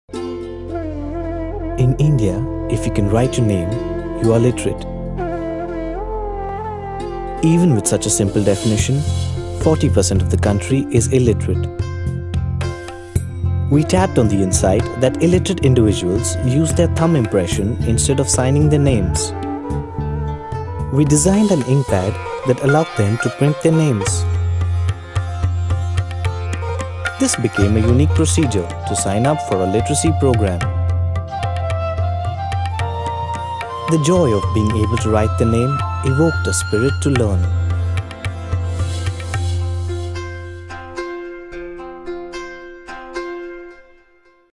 Inkpad 45 sec SILVER WINNER @ CANNES
This version was made to go along with the 2 min video for entries at the Cannes Lions 12'. has been shortlisted in 3 categories Direct Lions / Promo & Activation Lions / Media Lions Shortlist..

SILVER @CANNES - MEDIA LIONS